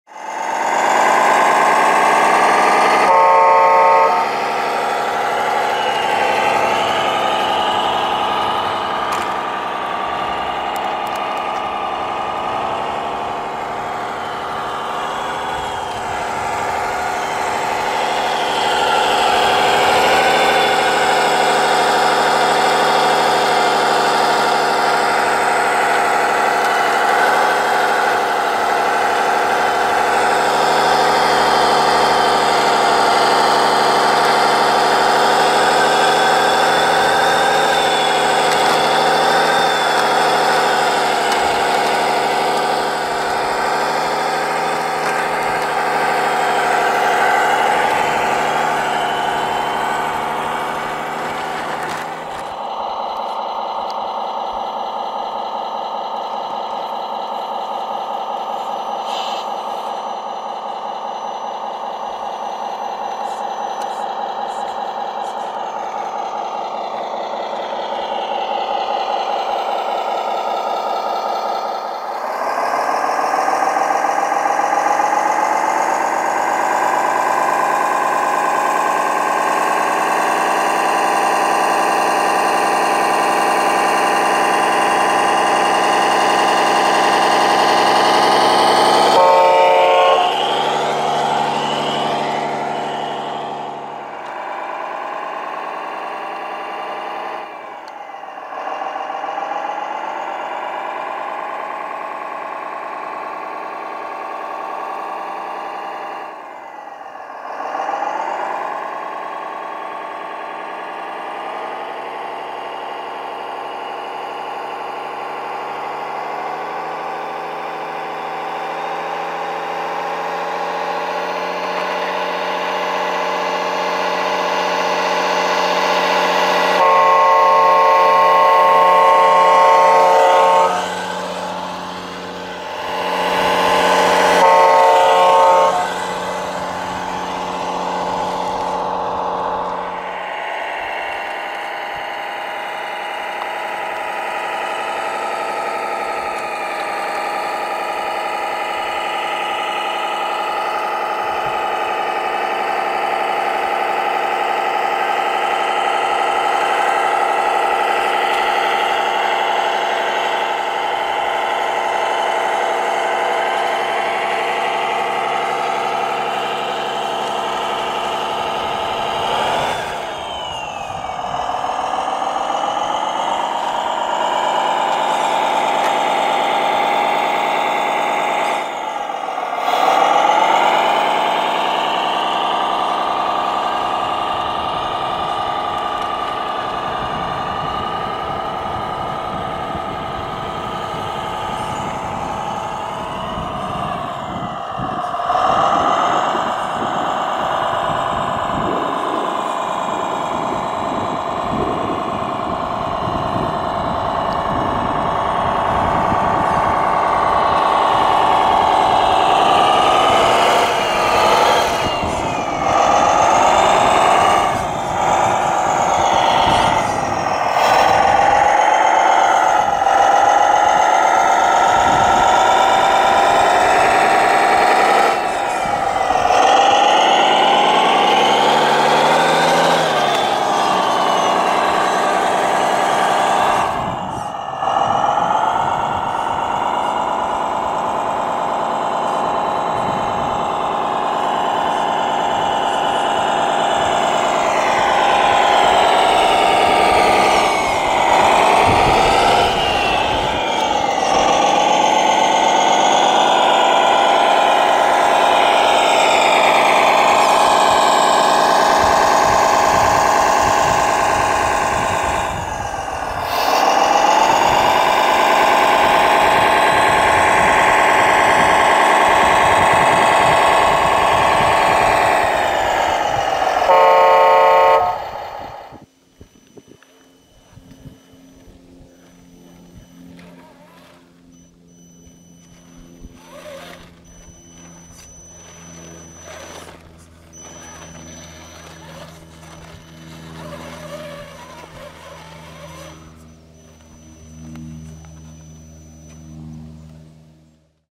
HC6 CROSS RC IN ACTION! AMAZING RC VEHICLES AT THE CONSTRUCTION ZONE! T-247 AND K-700 TRANSPORTATION
HC6 work so hard at the construction site. best rc vehicles in action at the real construction .